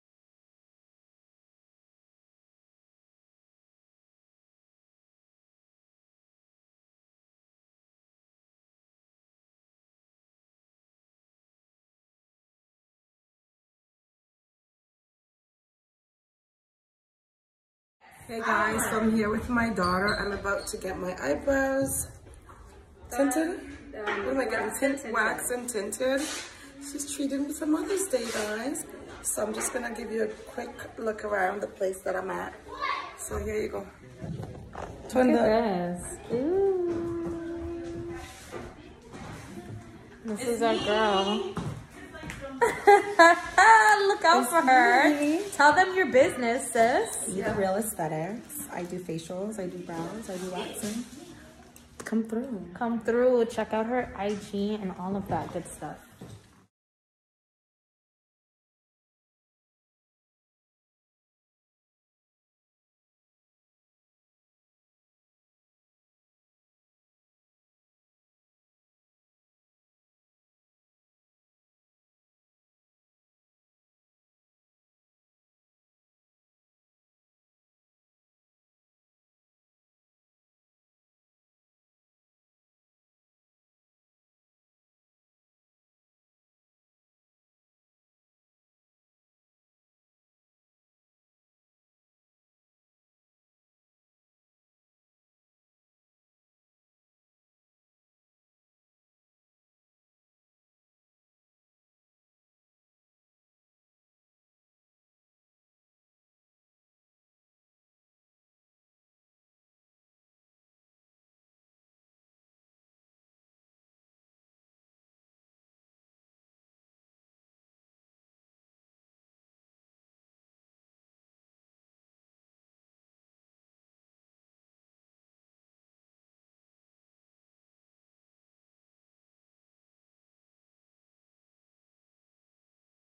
GETTING MY BROWS WAXED AND TINTED (MY DAUGHTER TREATED ME)
Hey my beautiful family, hope you’re having a wonderful day. The weather is absolutely gorgeous out. My daughter took me out to get my eyebrows done today, I so appreciate love and appreciate her. Also was done by her friend who’s a certified up and coming Aesthetician, thank you Jyzelle, you did an awesome job. Guys her website is under construction and will be up shortly.
Happy Mother’s Day to all my beautiful Queens near and far, God bless you all for the wonderful jobs you’re doing. Love you all